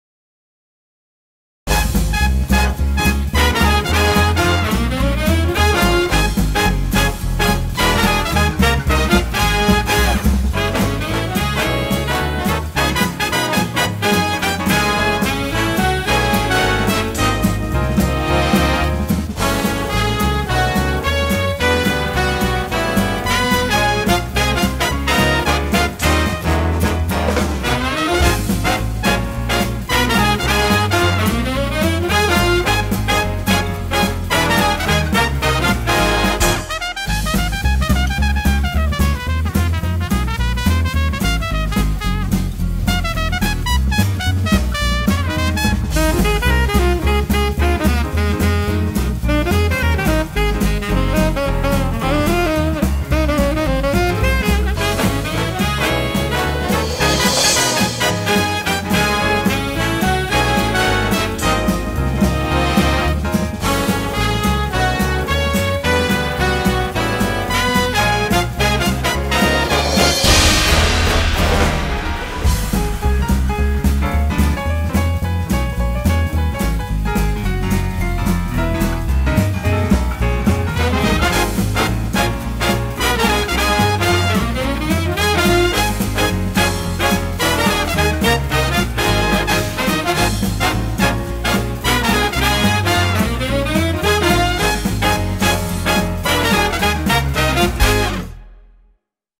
Fractal Design at CES 2012
CES 2012 montage video.   Paying tribute to the old school Vegas with a Rat Pack/Ocean's 11 feel...and sound.